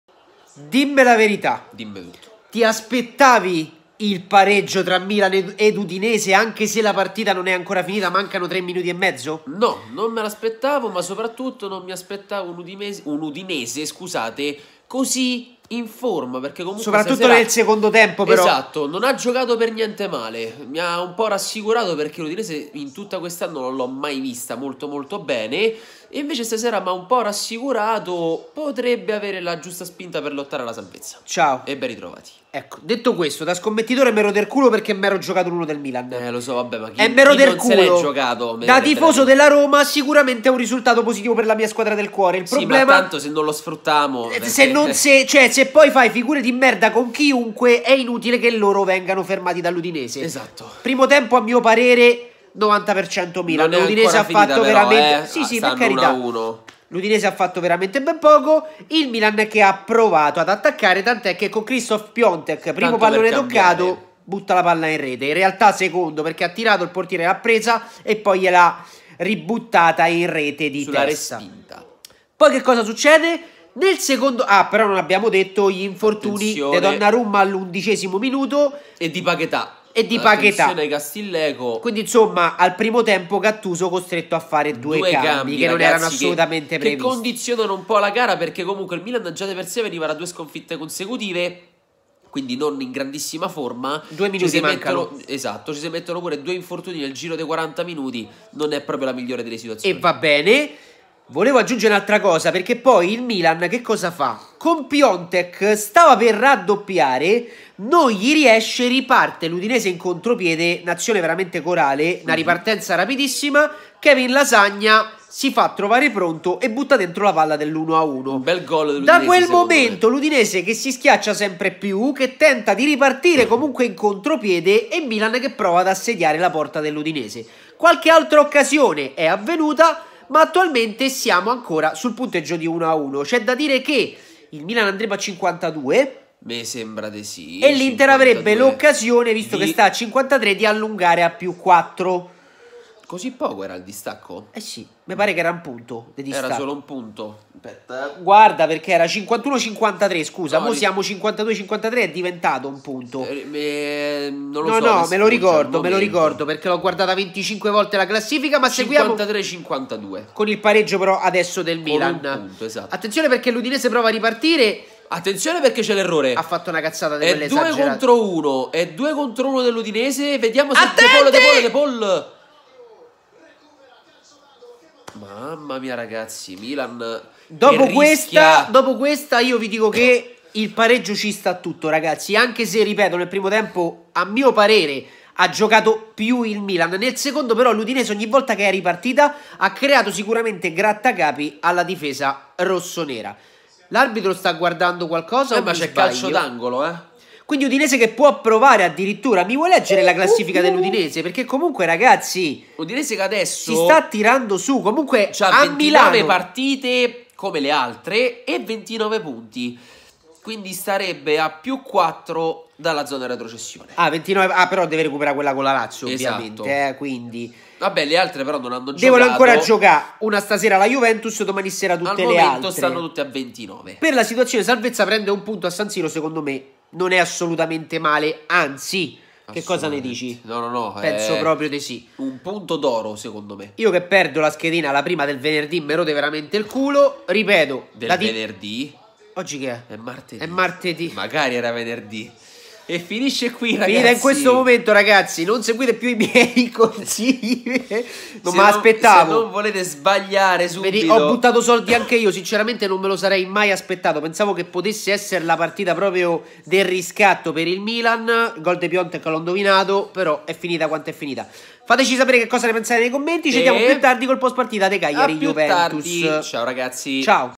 PIATEK NON BASTA: LASAGNA FERMA IL MILAN! Milan-Udinese 1-1
Piatek non basta: Lasagna ferma i rossoneri! Milan-Udinese 1-1